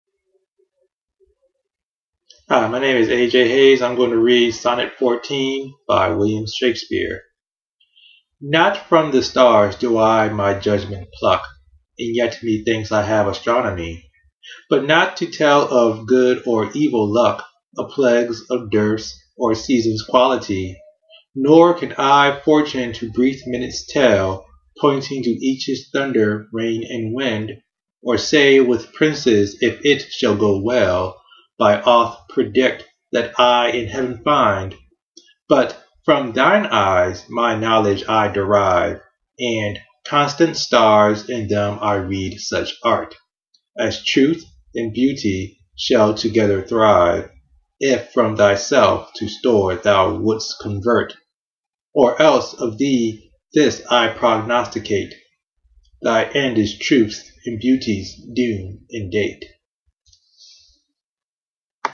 The Buzz 154 Project: Shakespeare's Sonnet #14
The Buzz 154 Project is an attempt to crowdsource short videos of people reading each and every one of Shakespeare's 154 sonnets. To learn more and join in the fun, visit BuzzBookstore.com